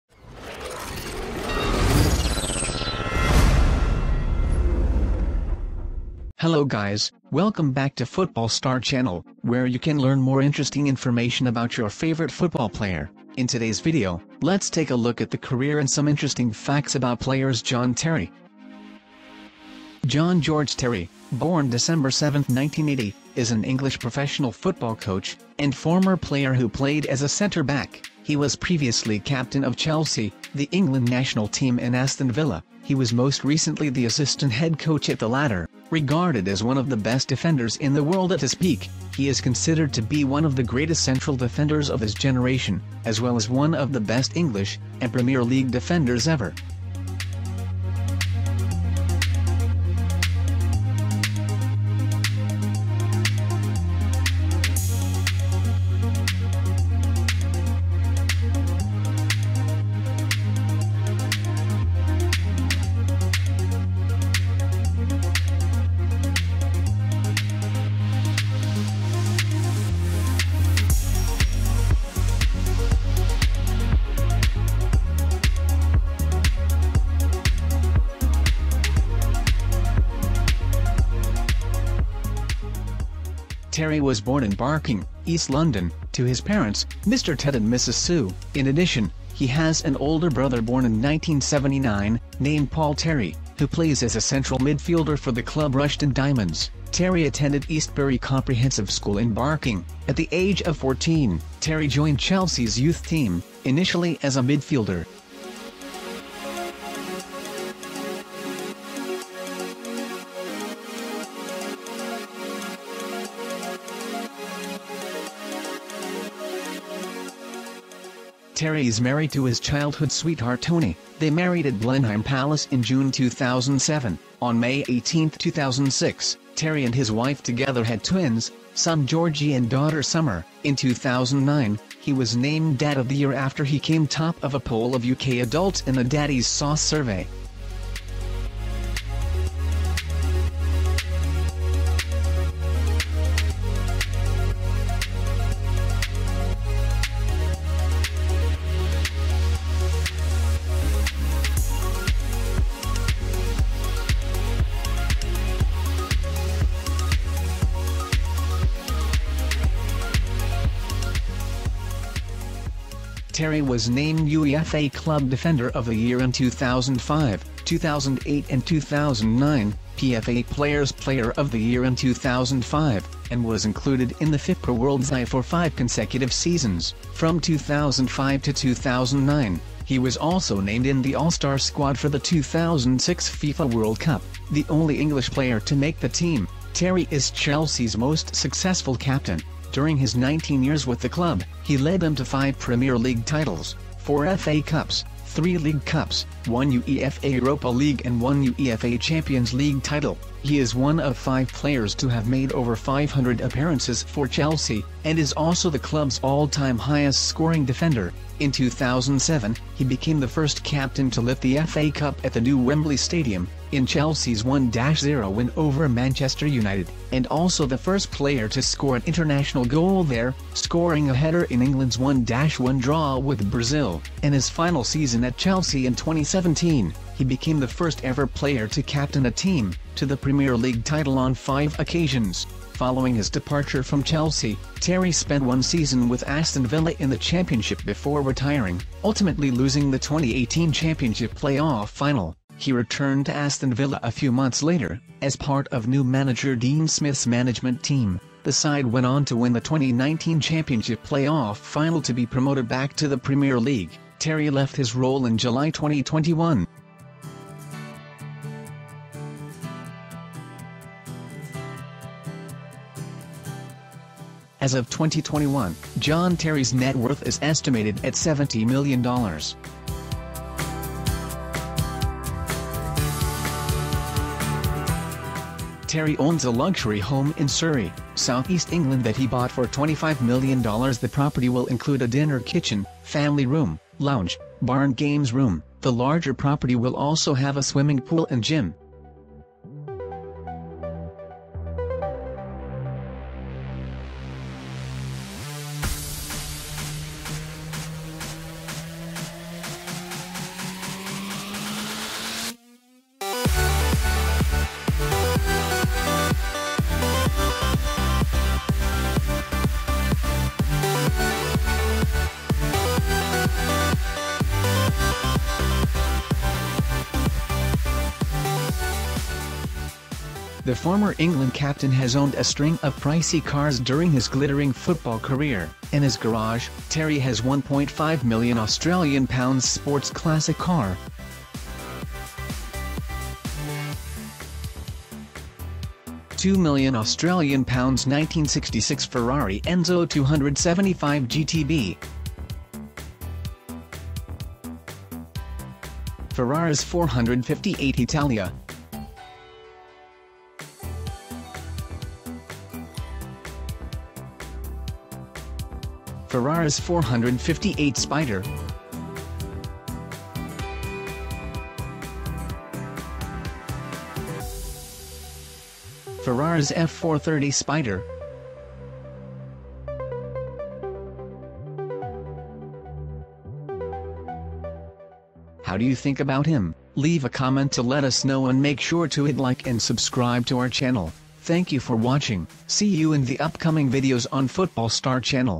John Terry's Lifestyle ⭐ 2021
John Terry.
Hello guys, welcome back to Football Star channel, where you can learn more interesting information about your favorite football player. In today's video, let's take a look at the career and some interesting facts about players John Terry.
John George Terry; (born 7 December 1980), is an English professional football coach, and former player who played as a centre-back. He was previously captain of Chelsea, the England national team, and Aston Villa. He was most recently the assistant head coach at the latter. Regarded as one of the best defenders in the world at his peak, he is considered to be one of the greatest central defenders of his generation, as well as one of the best English, and Premier League defenders ever.

source image, video:
John Terry (images)